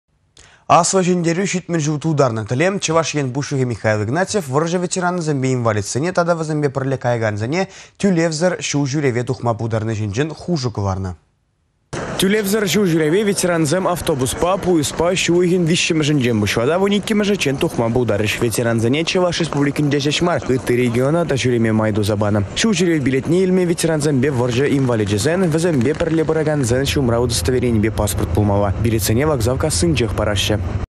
Ветерансене тÿлевсĕр çул çÿревпе тивĕçтерĕç
Аслă Çĕнтерÿ 70 çул тултарнă тĕле Чăваш Ен Пуçлăхĕ Михаил Игнатьев вăрçă ветеранĕсемпе инвалидсене тата вĕсемпе пĕрле каякансене тÿлевсĕр çул çÿреве тухма пултарни çинчен хушу кăларнă